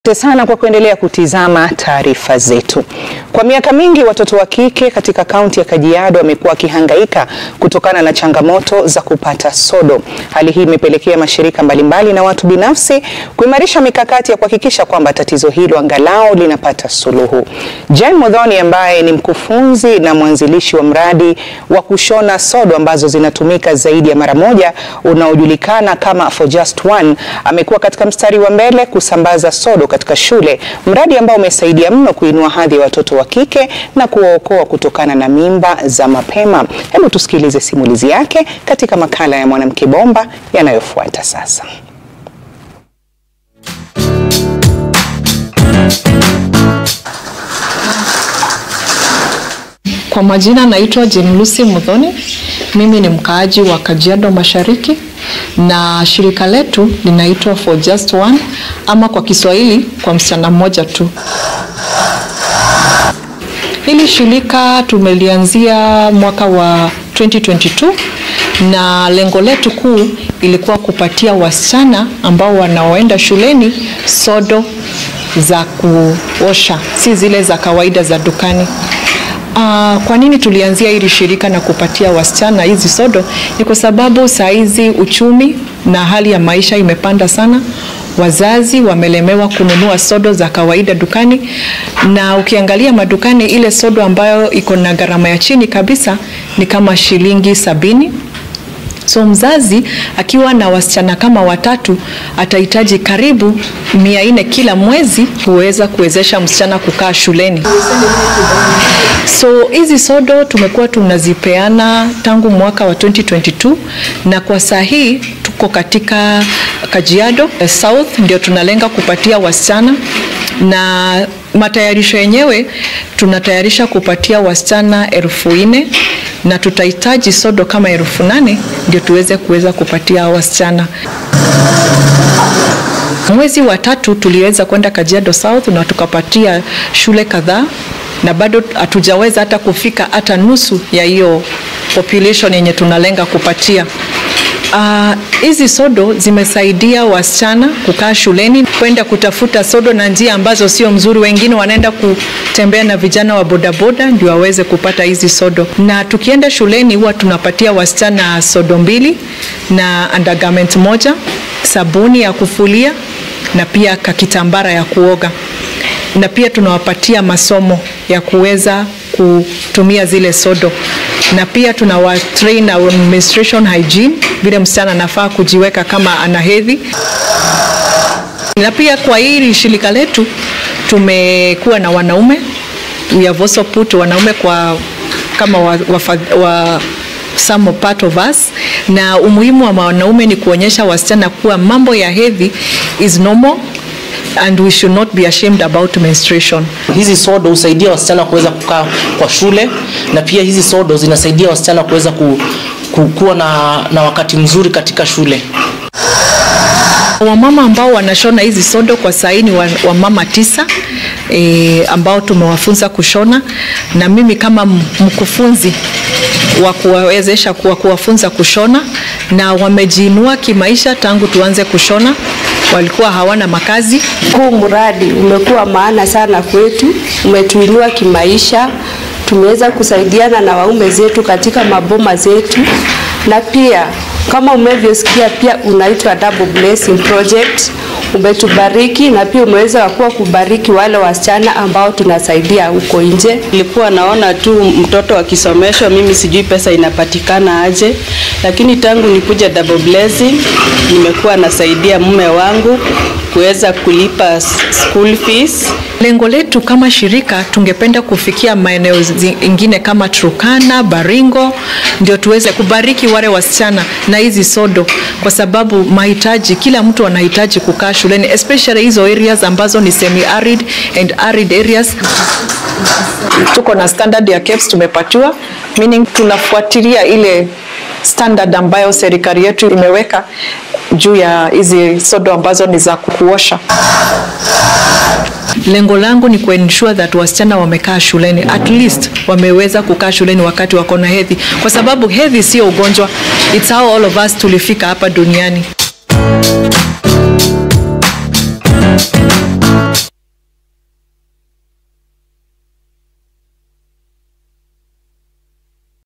Mwanamke Bomba | Mkufunzi wa kutengeneza sodo za kisasa
Sodo hizi hushonwa na kutumika zaidi ya maramoja